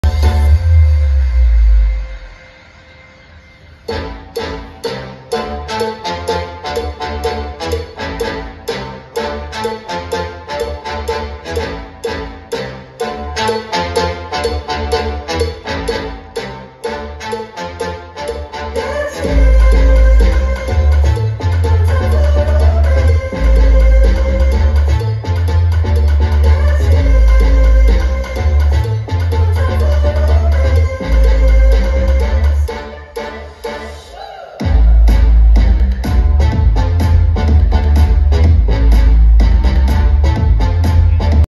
Intelligent lighting 6 tower example
Quick video of what intelligent lighting looks like